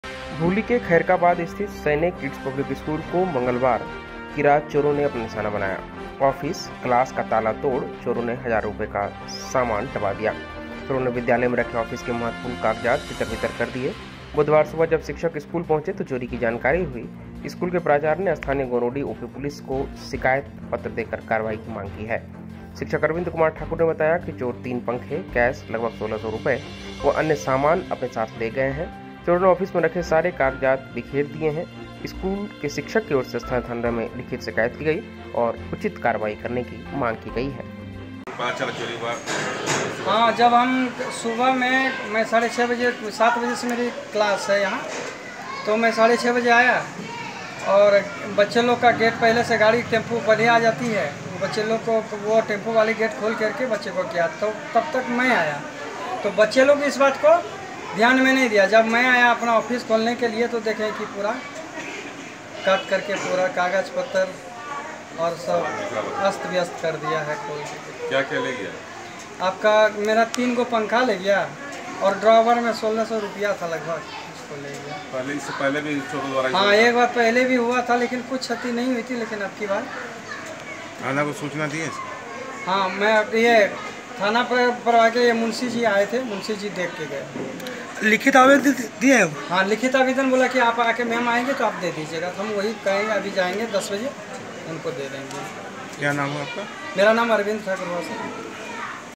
भूली के खैरकाबाद में स्कूल को चोरों ने बनाया निशाना
शैनय किड्स पब्लिक स्कूल का ताला तोड़ कर नकदी व सामानों पर फेरा हाथ